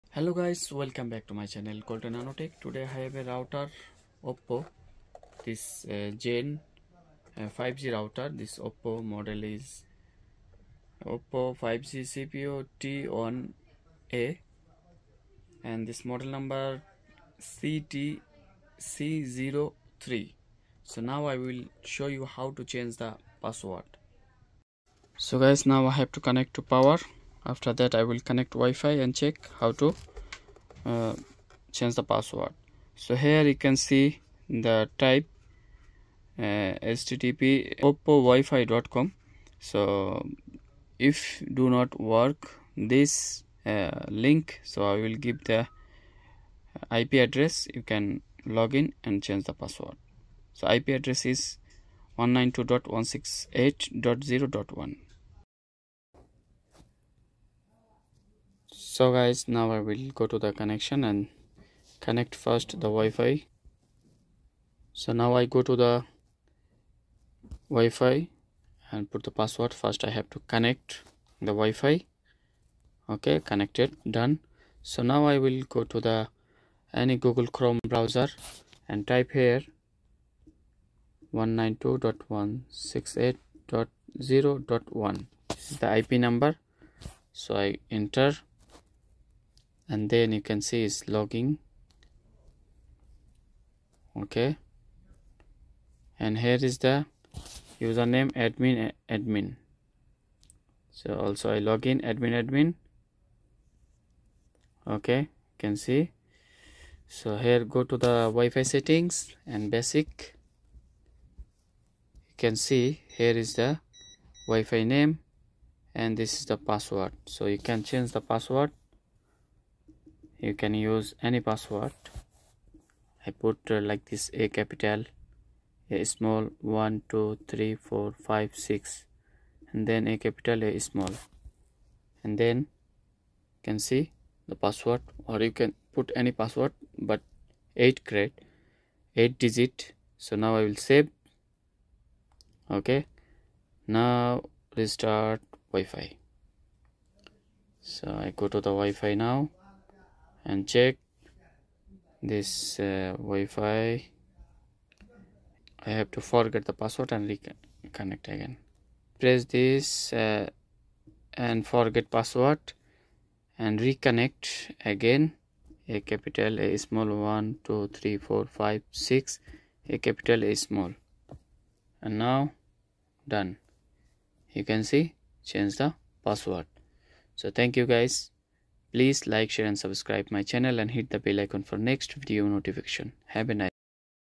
HOW TO CHANGE OPPO 5G ROUTER WIFI PASSWORD/oppo router ki password kaise change karna ha
কি ভাবে অপো রাউটার পাসওয়ার্ড পরিবর্তন করবেন।
how to change the password oppo 5G router at home